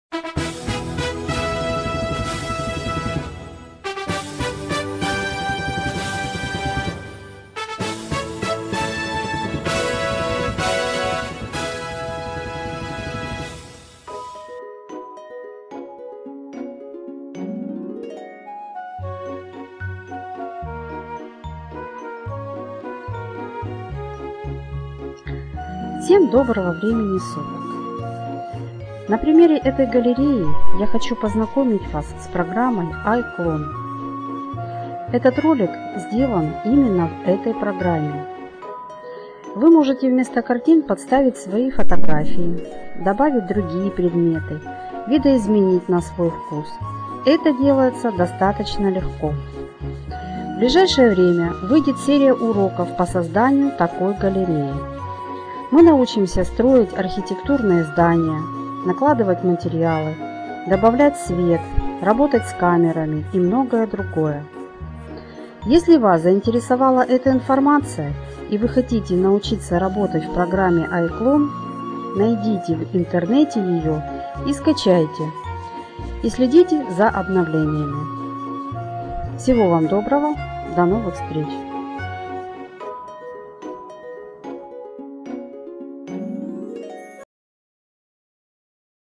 Картинная галерея/ Art Gallery
Некоторые возможности программы iClone. Если вы хотите научиться работать в этой программе, найдите ее в интернете, установите на свой компьютер и следите за обновлениями. Скоро выйдет серия уроков по программе iClone.